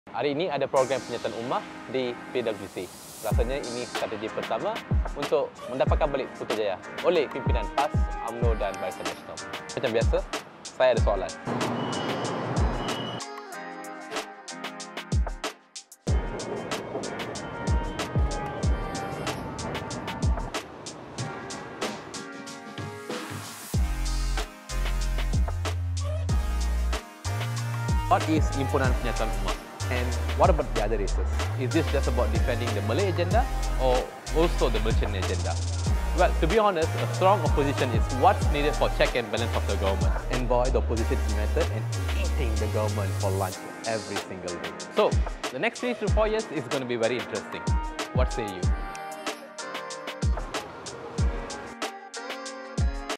HIMPUNAN PENYATUAN UMMAH
Ada apa dengan Himpunan Penyatuan Ummah?

Saya berada sekitar KL tadi, dan ambil masa seketika untuk menjenguk himpunan HPU914 yang dibuat di PWTC.

Vlog kali ini, saya beri ruang untuk ANDA berikan kupasan mengenai Himpunan ini dan impaknya kepada ruang politik Malaysia akan datang.

Saya rasa bagi banyak pemerhati, khususnya orang non-muslim dan diatas pagar, mahu tahu :
Apa itu Penyatuan Ummah?
Bagaimana pula dengan skop kaum lain?
Adakah ianya hanya mengenai menjamin dan menjaga Agenda Islam, atau menyeluruh kepada Agenda Malaysia?

I FOR ONE AM A BIG SUPPORTER OF A STRONG OPPOSITION, and nampaknya, pembangkang memang kuat, united, dan bertekad untuk memenangi semula Putrajaya.

So kenalah, ada better effort, and result from Kerajaan selepas nii. Is it possible? Maybe.

Obviously, sekarang too early to tell how things will play out. But, its going to be an interesting 2-3 tahun lagi for sure.

Boleh tolong SHARE atau LIKE ??

Nak aku kupas isu lain? Komen je dalam video..